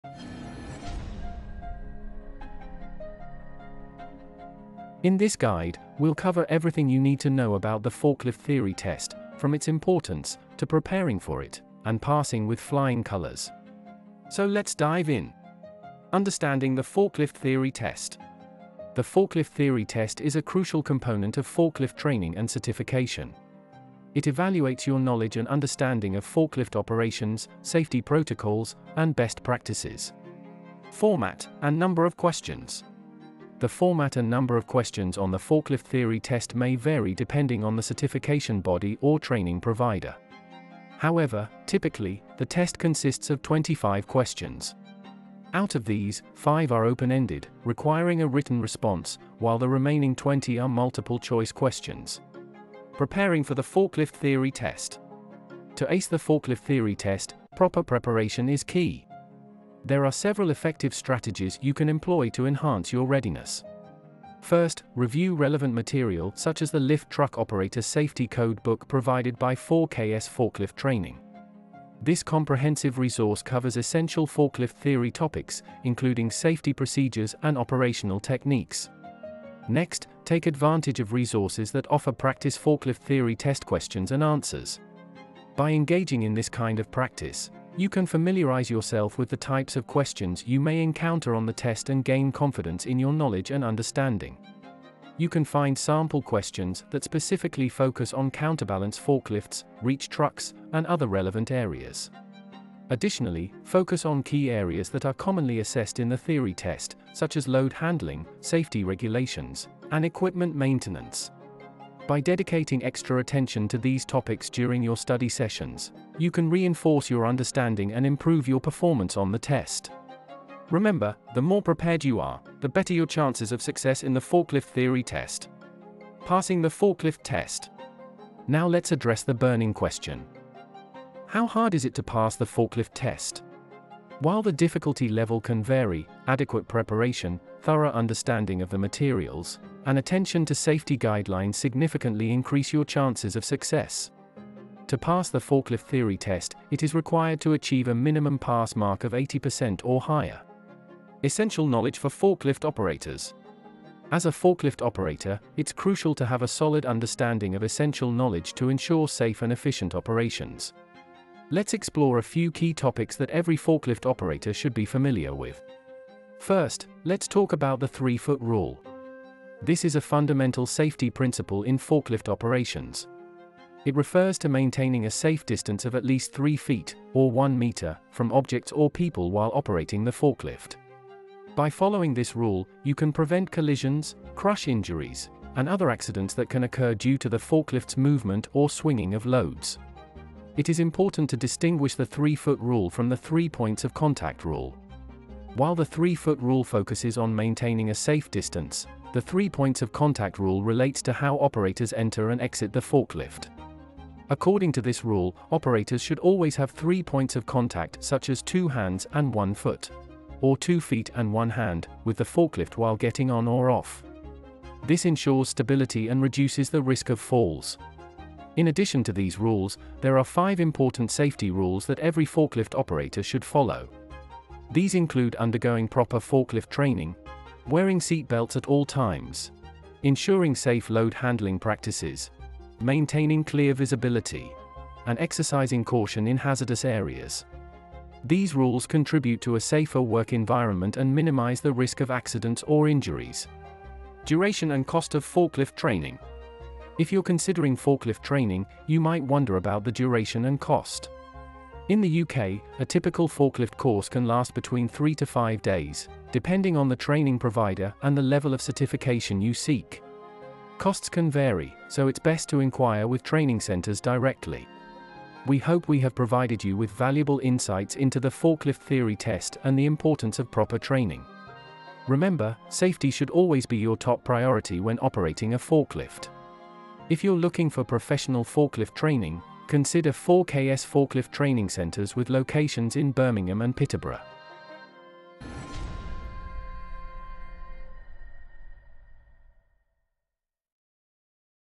Mastering the Forklift Theory Test | Essential Tips and Practice Questions | 4KS Forklift Training
Are you preparing for the forklift theory test? Discover essential tips, sample questions, and strategies to excel in the forklift theory test. Boost your knowledge on counterbalance forklifts, reach trucks, and more. Ace the test and become a certified forklift operator. Watch now and ensure your success!

0:00 - Introduction to Forklift Theory Test

In this guide, we'll cover everything you need to know about the forklift theory test, from its importance, to preparing for it, and passing with flying colours.

0:15 - Understanding the Forklift Theory Test

The forklift theory test is a crucial component of forklift training and certification. It evaluates your knowledge and understanding of forklift operations, safety protocols, and best practices.

0:30 - Format, and Number of Questions

The format and number of questions on the forklift theory test may vary depending on the certification body or training provider. However, typically, the test consists of 25 questions. Out of these, 5 are open-ended, requiring a written response, while the remaining 20 are multiple-choice questions.

0:53 - Preparing for the Forklift Theory Test

To ace the forklift theory test, proper preparation is key.
There are several effective strategies you can employ to enhance your readiness.

First, review relevant material such as the lift truck operator safety code book provided by 4KS Forklift Training. This comprehensive resource covers essential forklift theory topics, including safety procedures and operational techniques.

1:20 - Forklift Theory test Questions and Answers

Next, take advantage of resources that offer practice forklift theory test questions and answers.
By engaging in this kind of practice, you can familiarise yourself with the types of questions you may encounter on the test and gain confidence in your knowledge and understanding.
You can find sample questions that specifically focus on counterbalance forklifts, reach trucks, and other relevant areas.

Additionally, focus on key areas that are commonly assessed in the theory test, such as load handling, safety regulations, and equipment maintenance.
By dedicating extra attention to these topics during your study sessions, you can reinforce your understanding and improve your performance on the test. Remember, the more prepared you are, the better your chances of success in the forklift theory test.

2:05 - Passing the Forklift Theory Test

How hard is it to pass the forklift test?
While the difficulty level can vary, adequate preparation, thorough understanding of the materials, and attention to safety guidelines significantly increase your chances of success. To pass the forklift theory test, it is required to achieve a minimum pass mark of 80% or higher.

2:30 - Essential Knowledge for Forklift Operators

As a forklift operator, it's crucial to have a solid understanding of essential knowledge to ensure safe and efficient operations.
Let's explore a few key topics that every forklift operator should be familiar with.

2:45 The "3-Foot Rule" in Forklift Safety
This is a fundamental safety principle in forklift operations.
It refers to maintaining a safe distance of at least three feet (or one metre) from objects or people while operating the forklift.
By following this rule, you can prevent collisions, crush injuries, and other accidents that can occur due to the forklift's movement or swinging of loads.

3:15 - The 3 points of contact rule

While the 3 foot rule focuses on maintaining a safe distance, the three points of contact rule relates to how operators enter and exit the forklift.
According to this rule, operators should always have three points of contact such as two hands and one foot  or two feet and one hand, with the forklift while getting on or off.

3:40 - Five Important Safety Rules for Forklift Operators
In addition to these rules, there are five important safety rules that every forklift operator should follow

These include undergoing proper forklift training,  wearing seat belts at all times,  ensuring safe load handling practices,  maintaining clear visibility,  and exercising caution in hazardous areas.

These rules contribute to a safer work environment and minimise the risk of accidents or injuries.

4:03 - Duration and Cost of Forklift Training

If you're considering forklift training, you might wonder about the duration and cost.
In the UK, a typical forklift course can last between 3 to 5 days, depending on the training provider and the level of certification you seek. Costs can vary, so it's best to inquire with training centres directly.

We hope we have provided you with valuable insights into the forklift theory test and the importance of proper training. Remember, safety should always be your top priority when operating a forklift.

Code: OCCEFLWVEOFZQBUE